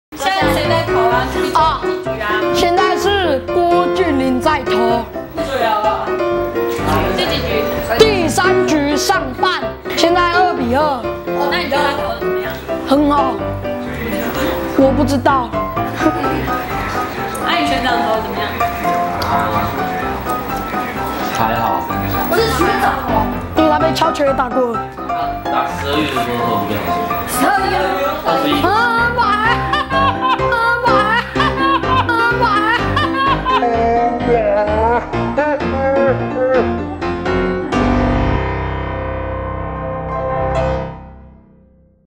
【2015中華隊專輯】U12花絮 - 蝦米?吳秉恩想徵召學長郭俊麟打12U?
U12熱鬧開打，首戰中華小將擊敗韓國，集訓期間他們是怎麼培養感情呢？為什麼旅日郭俊麟會被徵召打12U呢？請看影片！不要笑太大聲喔！
‪#‎U12‬ ‪#‎中華隊‬ ‪#‎vamos‬